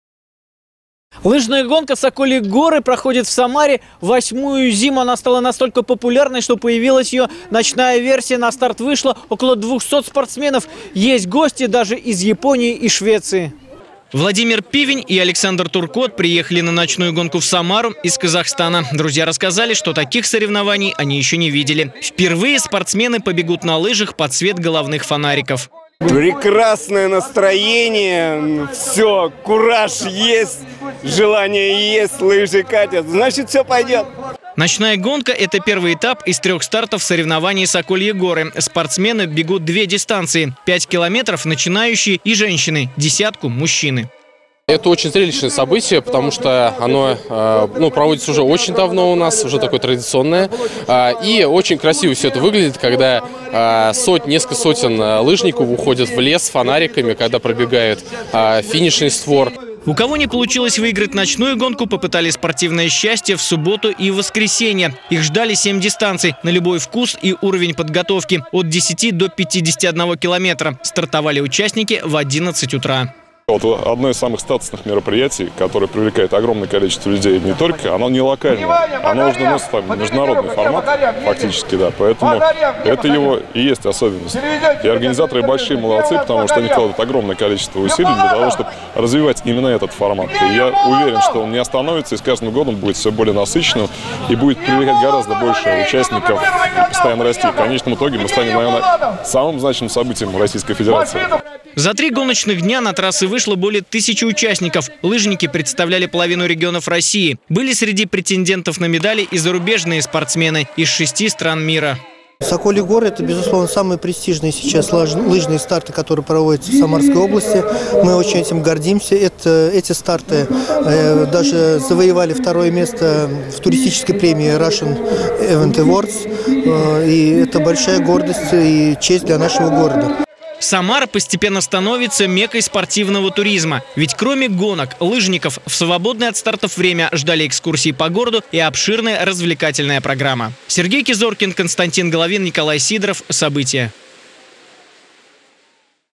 Марафон "Сокольи горы" собрал лыжников со всего мира
Спортивный туризм докатился и до Самары. Более тысячи лыжников со всего света приехали на гонку "Сокольи горы". Было три старта. Один из них прошел после захода солнца. Под фонарями и звездами экстремалов увидел Сергей Кизоркин.